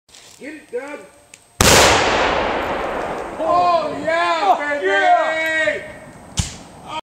bowling ball launch
16 lb. bowling ball and a little over 1/4 cup black powder. About 70 feet straight up